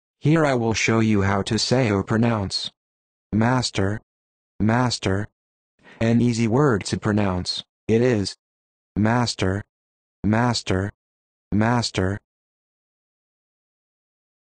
How to Say or Pronounce Master
This video shows you how to say or pronounce Master.

How accurate does it say Master? How would you say Master?